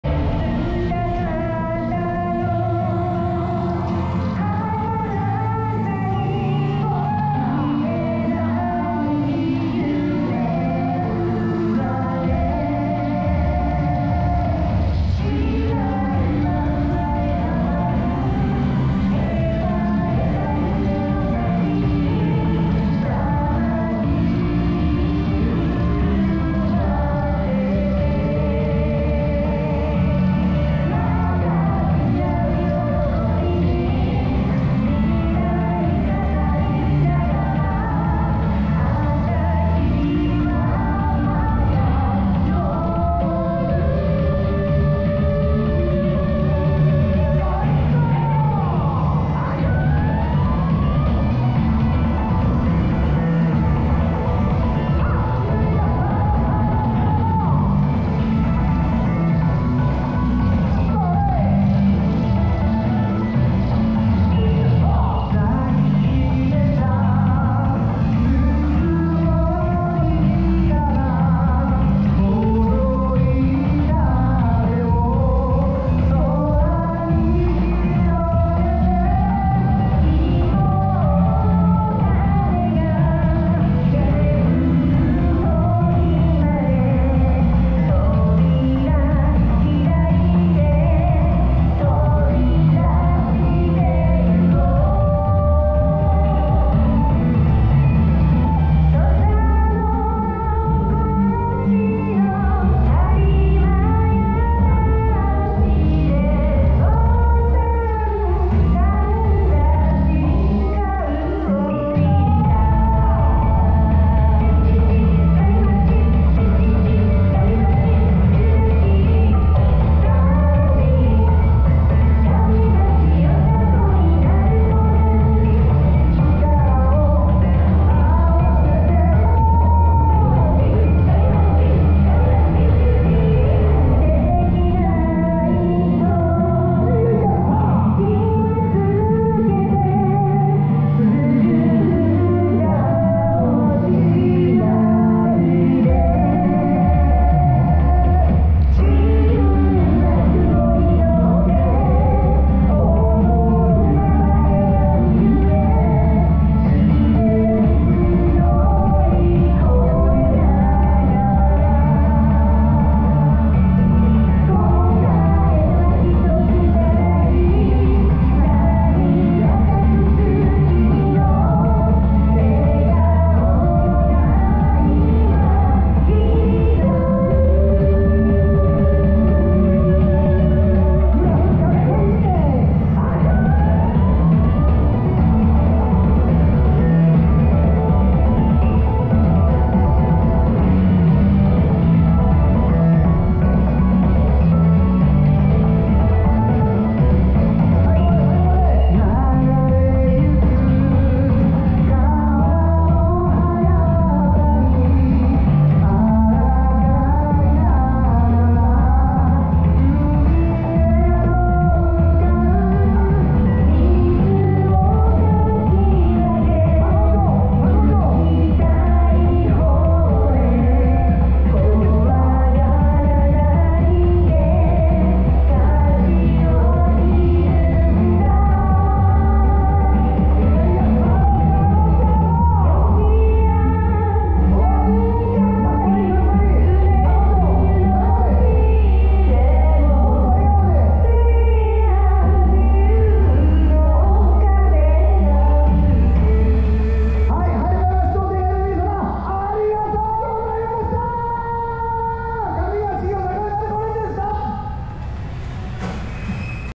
2024/08/10[隊列後方·途中から]･はりまや橋競演場･第71回よさこい祭･上町よさこい鳴子連
撮影場所　高知市（はりまや橋競演場）。